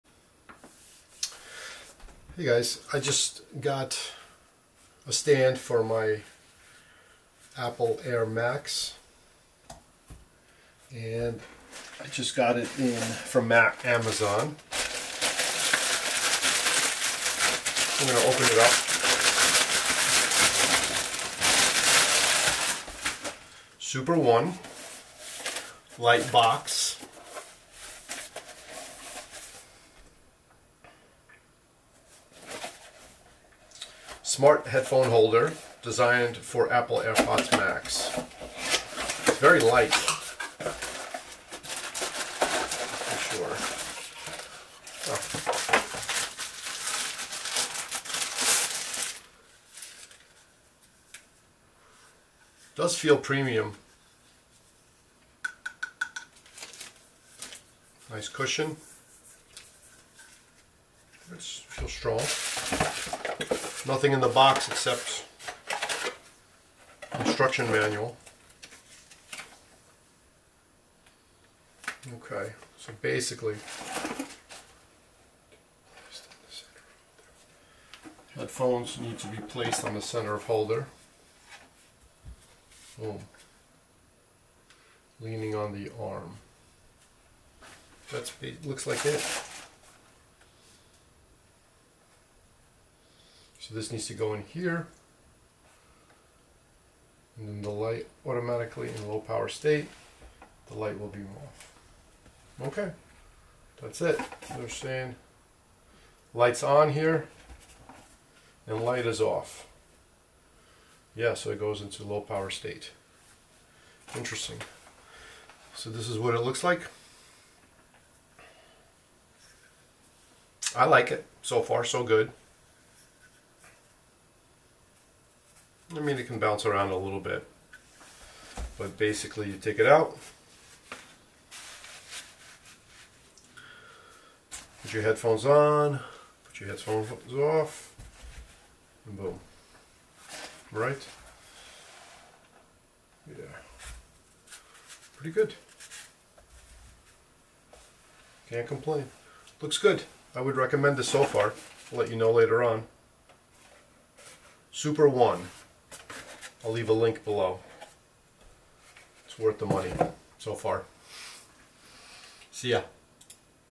🎧 My New Favorite Accessory! SUPERONE Smart Headphone Stand for AirPods Max - Unboxing and Review
LINK TO PRODUCT

I'm so excited to share my new favorite accessory with you! Today, we're unboxing and reviewing the SUPERONE Smart Headphone Stand designed specifically for the New Apple AirPods Max. This aluminum alloy headphone stand is not only sleek and stylish, but it's also practical! With a hibernating base and Apple Smart Low-power Technology, you can rest assured that your AirPods Max are in safe hands. The one-handed operation makes it so easy to use, and I love the soft microfiber cloth on the holder which provides comprehensive protection for my expensive headphones - no scratch damage, and easy to pick up! 🤩

In this video, we'll go over all the features of the product and share my personal experience using it. I love how unique the design is and how it holds my AirPods Max so firmly on the desk, I never have to worry about them tipping over. And if you have any problems or questions, SUPERONE provides an 18-month money-back guarantee or new replacements, and their friendly and helpful customer service team is always available to assist you. 👌

If you're like me and always looking for ways to elevate your tech setup, then you won't want to miss out on this amazing headphone stand. It's definitely a game-changer and I highly recommend it to anyone who wants to keep their AirPods Max safe and stylish! So, let's dive into this unboxing and review together and see if the SUPERONE Smart Headphone Stand is the right fit for you! 💯`

LINK TO PRODUCT